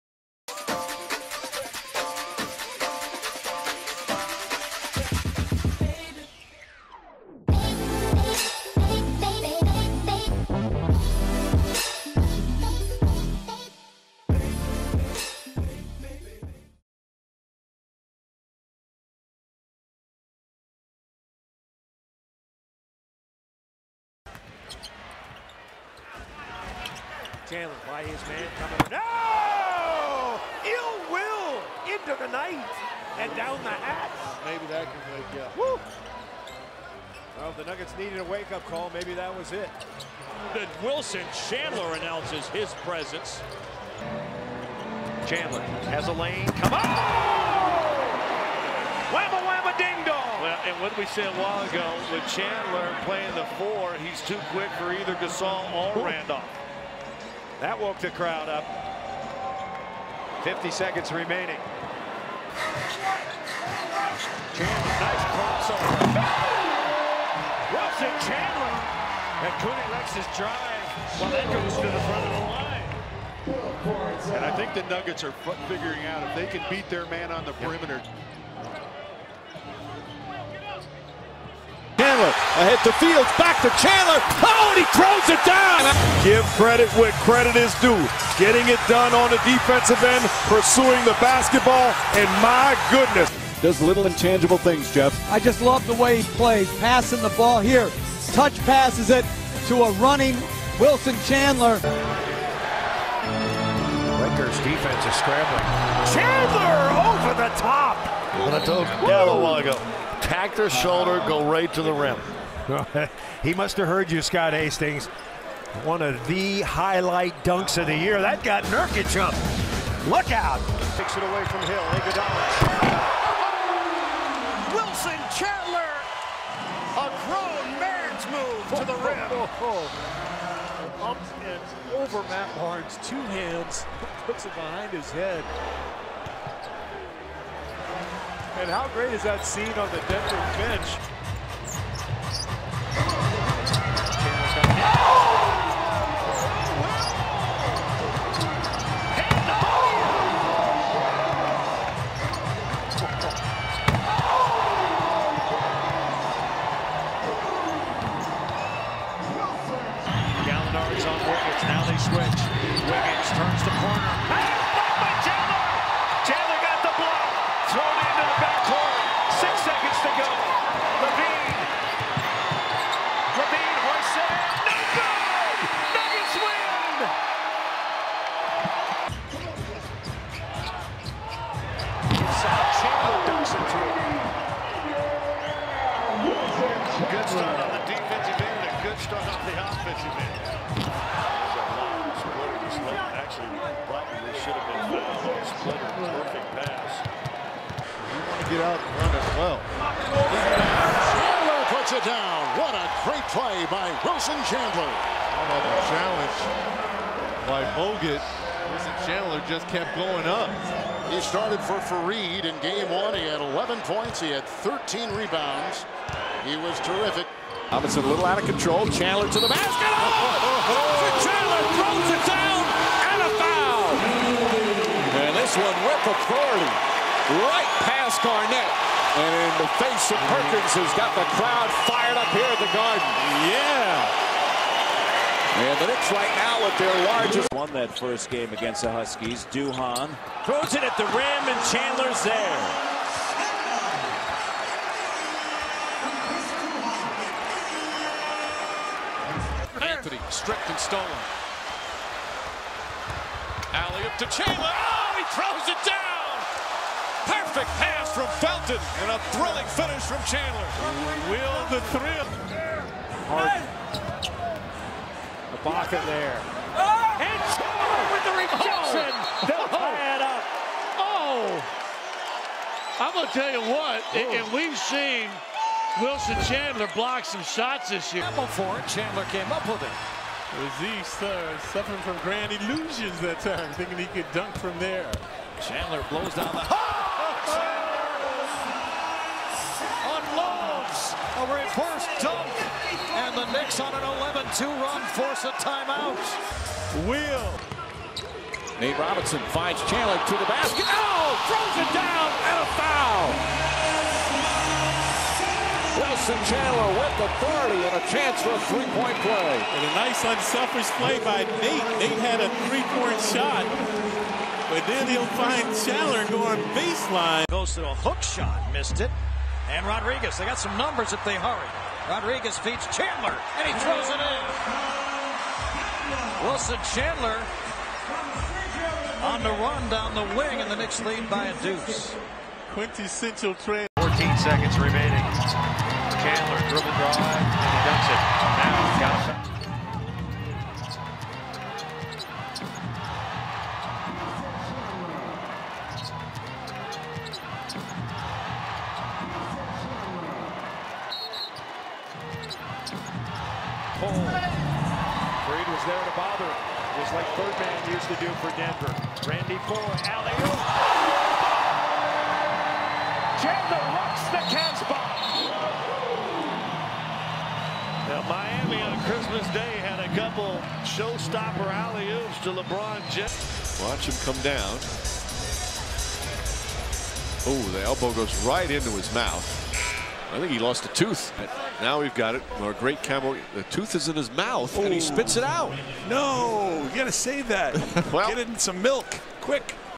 NBA Underrated Player: Wilson Chandler
A great guard for the Denver Nuggets who is not only versatile but can put up 16 points a game easily.